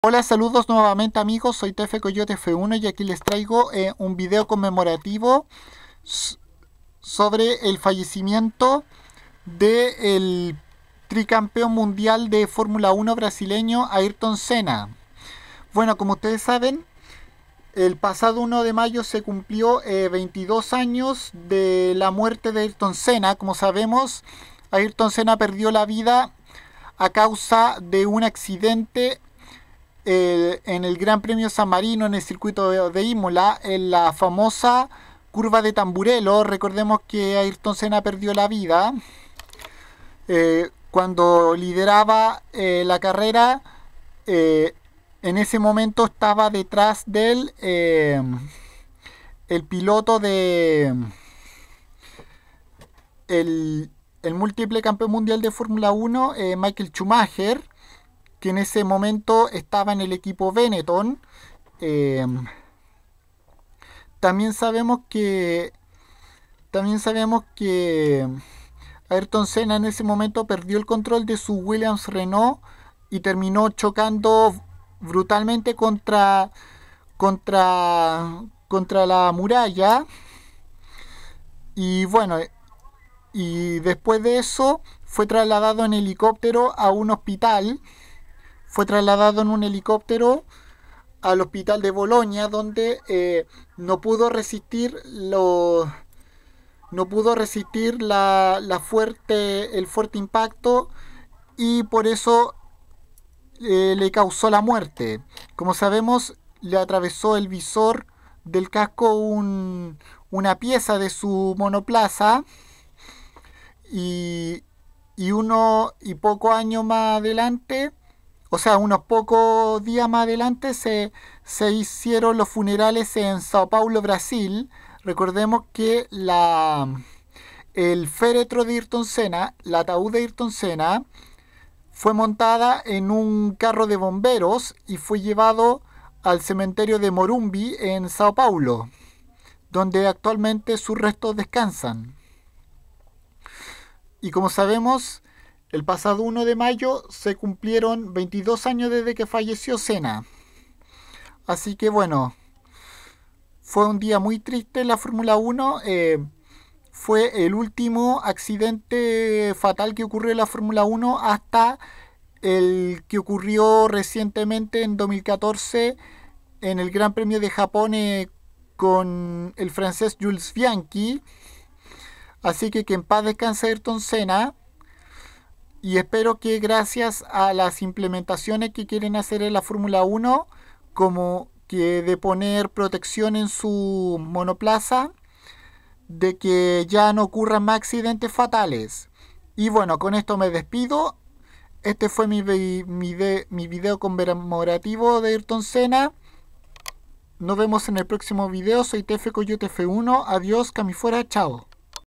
22 años de la muerte de Ayrton Senna
El pasado 1 de Mayo se conmemoraron 22 años desde que el piloto brasilero tri-campeón de F1 Ayrton Senna perdió la vida a causa de un terrible accidente en la famosa curva Tamburello en el Circuito de Imola durante el GP de San Marino del 94

Este es mi vídeo conmemorativo por este hecho